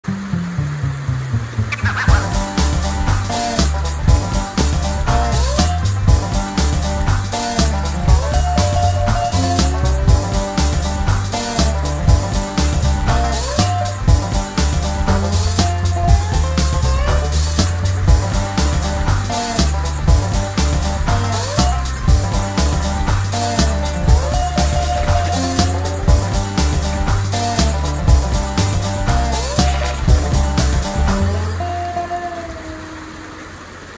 Southeast Alaska Garlic Dance Extended Version
A post-planting garlic dance in the rain.